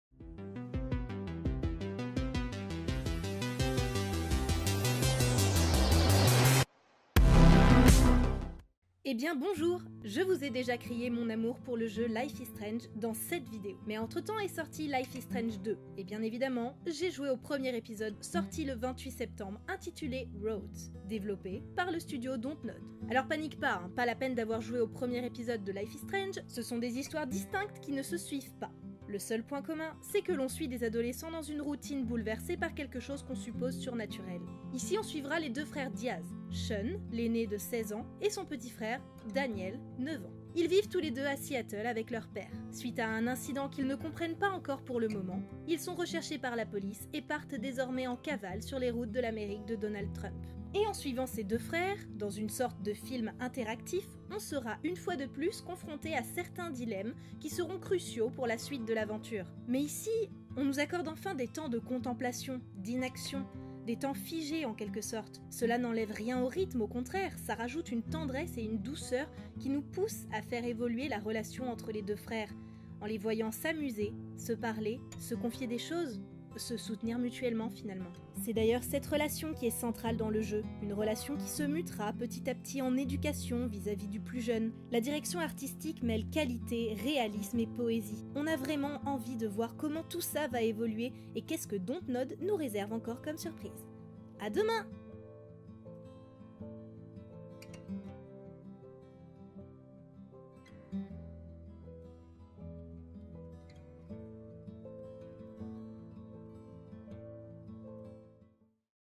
Calendrier des coups de coeur - Ep 5 Life is Strange 2
Hey Hey ! J'ai beaucoup aimé le premier épisode de Life Is Strange 2 et je n'ai pas pu résister à t'en parler :)
Hâte que le 2nd épisode sorte en janvier !! Big Up Dontnod!